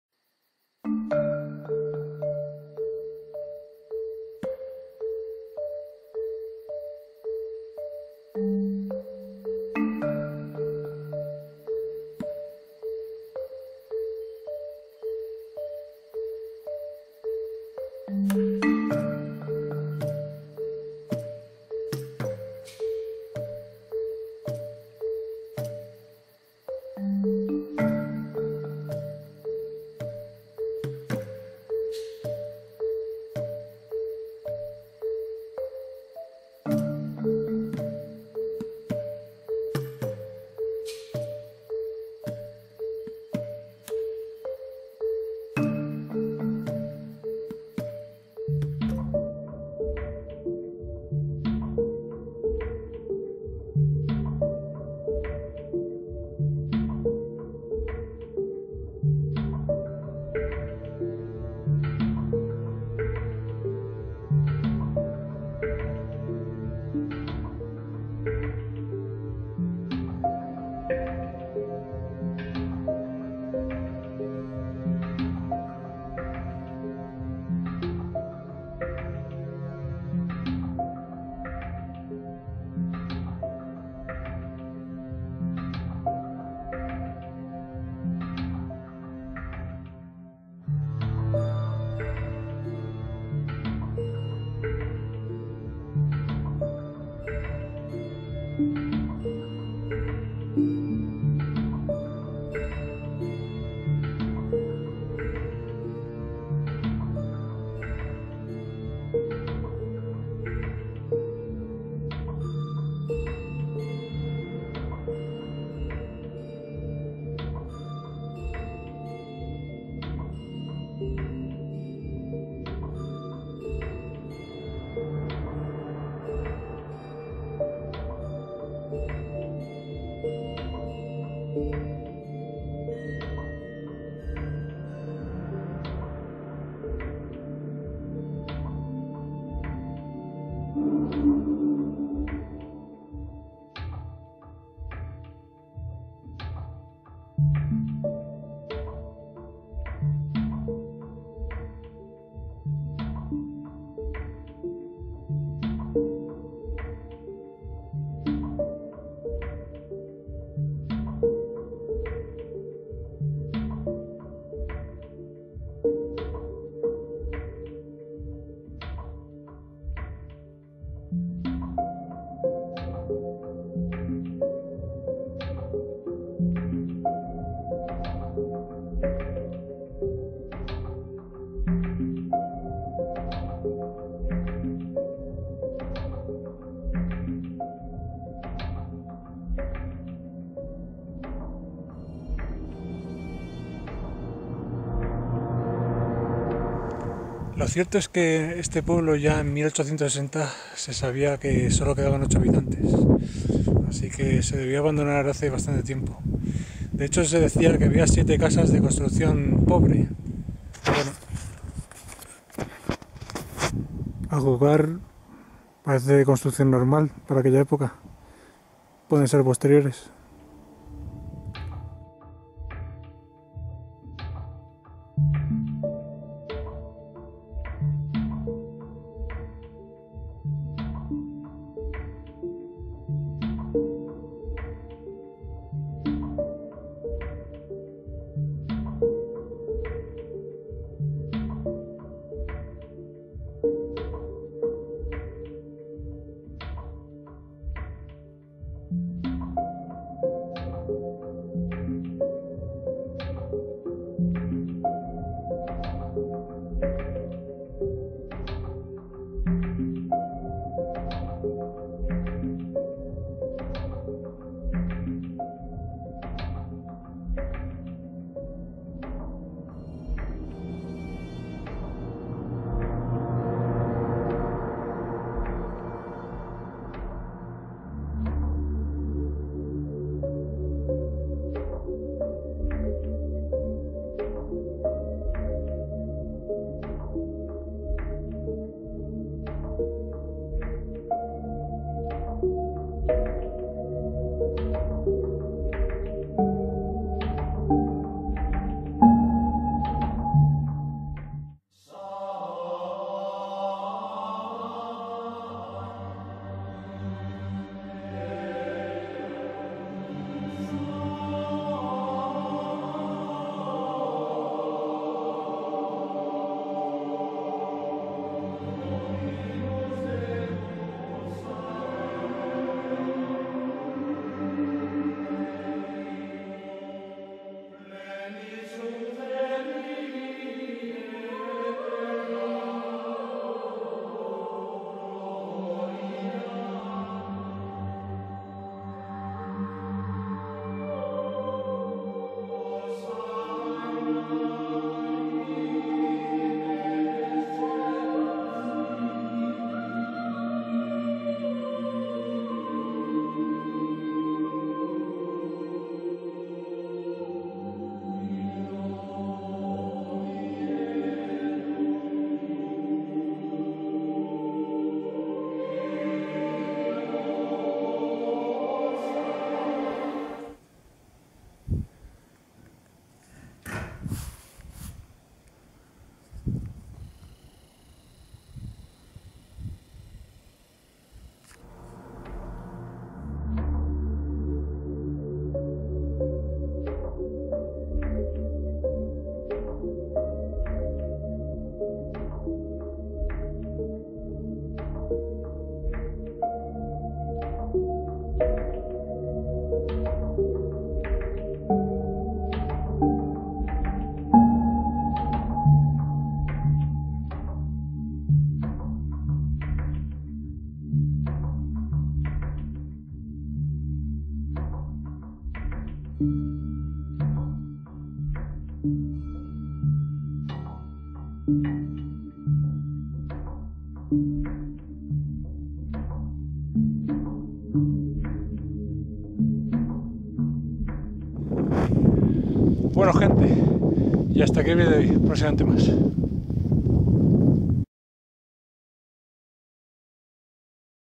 Explorando Pueblo Abandonado de Albocabe (Soria)
Buenas mi gente, aquí os traigo una nueva exploración en un pueblo abandonado. Es un pueblo pequeño pero muy interesante tanto en la actualidad como la historia que tiene. El día que fui era lluvioso y fue una verdadera odisea llegar hasta el pueblo por el camino lleno de charcos y barro pero mereció la pena, como veréis se muestra un ambiente ideal para explorarlo. Asi que sin mas os mando saludos a todos y mas información en la descripción:

 Albocabe es un despoblado perteneciente a Aliud en la provincia de Soria.
Con topónimo de origen árabe, se trata de un señorío perteneciente a Joaquín Carrillo, vecino de Vinuesa según el Catastro de la Ensenada. Por aquellas épocas, siglo XVIII, solamente 2 vecinos habitaban el poblado y la mayoría de los edificios se encontraban en ruinas.
Posteriormente, en 1860, Albocabe aparece como habitado por 8 vecinos y 7 casas de pobre construcción. Albocabe sin embargo, se muestra rodeado de manantiales y exquisitas aguas que daban lugar a viñedos, olivares y algún arbolado siendo así, un terreno fértil.
A mediados del siglo XIX, como relata el blog de Soria-Goig.com, se comenta que la iglesia dedicada a San Miguel estaba ya en ruinas. Aún así, pueden observarse algunos ornamentos e inscripciones en el interior de la iglesia.
Estación de ferrocarril: Localidad hoy deshabitada, en su día llegó a albergar una estación e instalaciones ferroviarias que formaban parte de la línea férrea Santander-Mediterráneo, entre los municipios de Gómara y Almenar de Soria.

Y hasta aquí toda la información del lugar, saludos de Explorador Solitario.